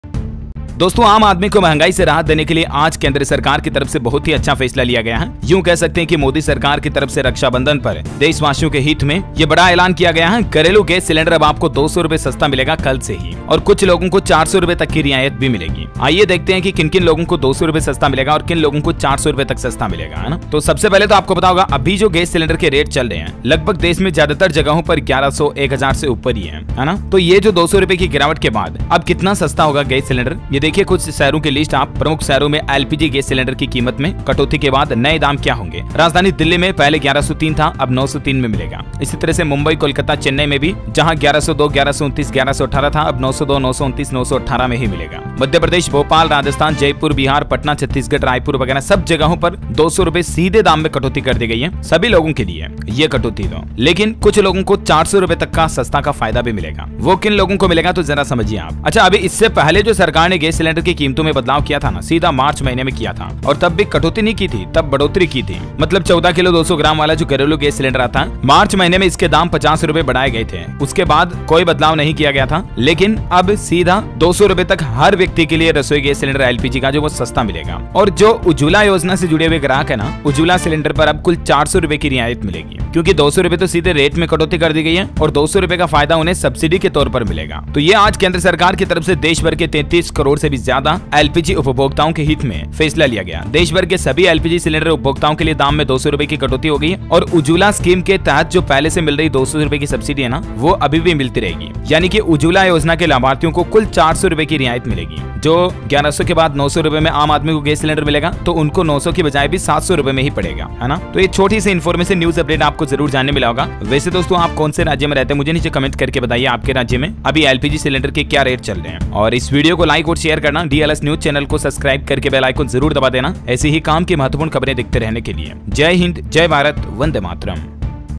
रक्षाबंधन पर मोदी सरकार का बड़ा तोहफा, LPG रसोई गैस सिलेंडर 400 रुपए तक सस्ता मिलेगा PM Modi news
रक्षाबंधन पर मोदी सरकार का बड़ा तोहफा, उज्ज्वला योजना के तहत 400 रुपए सस्ता हुआ रसोई गैस सिलेंडर

घरेलू LPG सिलेंडर 200 रुपये सस्ता, रक्षाबंधन से पहले मोदी सरकार का बड़ा तोहफा

घरेलू गैस सिलेंडर कल से ₹200 सस्ता: भोपाल में ₹908, जयपुर में ₹906 हुई कीमत; उज्ज्वला सिलेंडर पर अब कुल ₹400 की रियायत

200 की छूट किसे मिलेगी, 400 की किसे? रसोई गैस पर राहत से जुड़े हर सवाल का जवाब जानिए